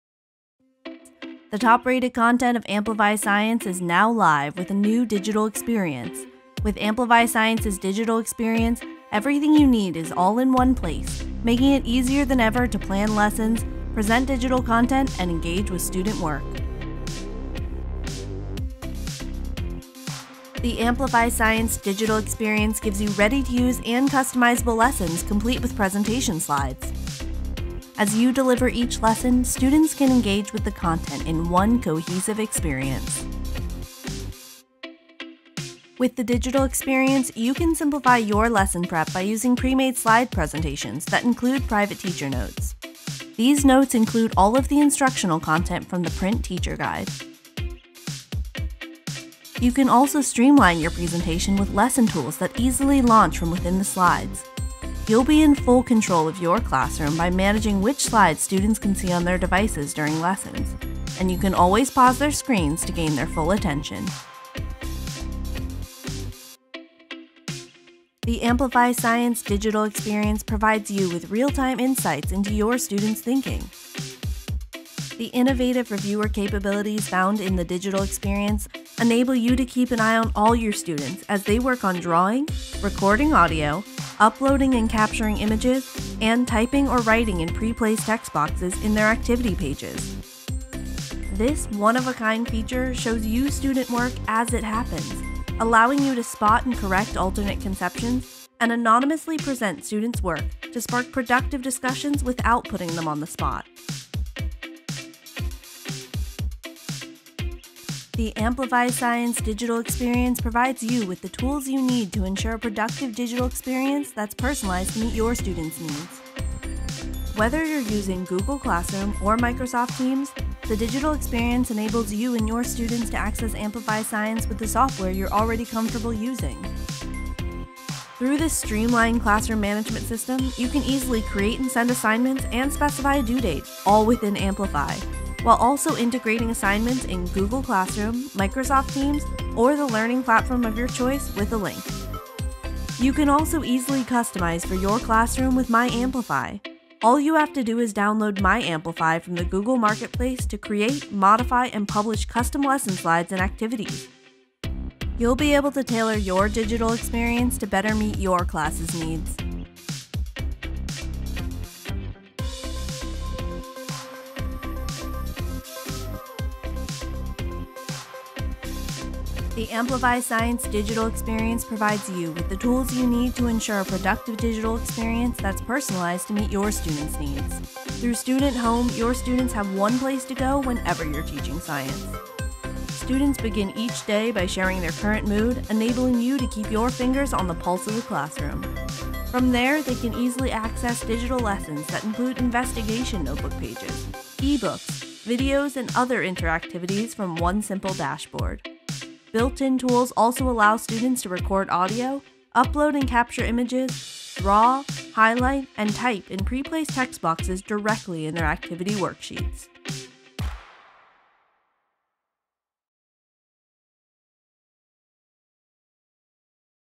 The Amplify Science digital experience I Amplify
The top-rated content of Amplify Science is now live with a new robust digital experience that enhances instruction and saves you time. Everything you need is in one place, making it easier than ever to plan lessons, present digital content, and review student work.